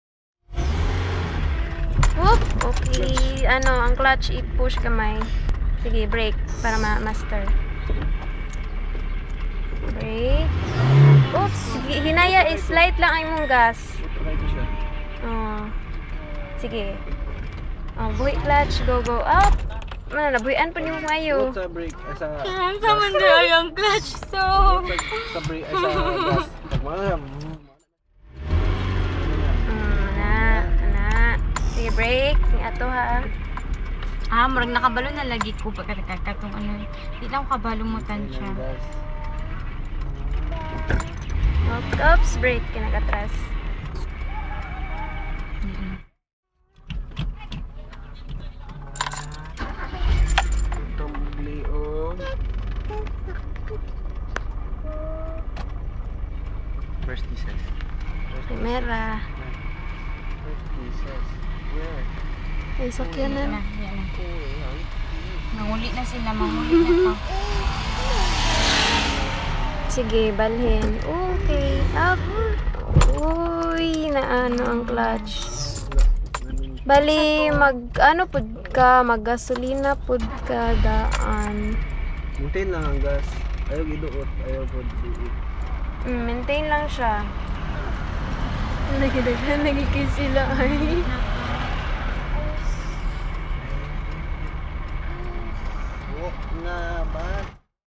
Adora's Driving Tutorial by Saomilk- Day 1
Sao & Milk taught their barkada, Adore how to drive manually. Refresher ra ni hah! hehe